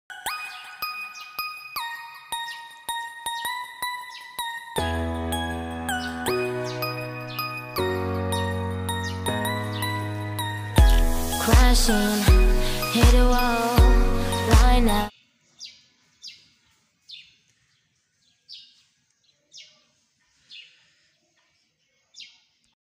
Preserving BSF larvae, the high quality protein source for fish, poultry, pigs etc
Black soldier fly larvae is high proteinous source of feed for poultry, fish, piggery and even ducks.
I use sun-drying to preserve the larvae and keep it for a longer time.
Black soldier fly larvae drying is simple as it involves harvesting them and then cleaning the larvae with hot water to kill them and have them clean before sun drying.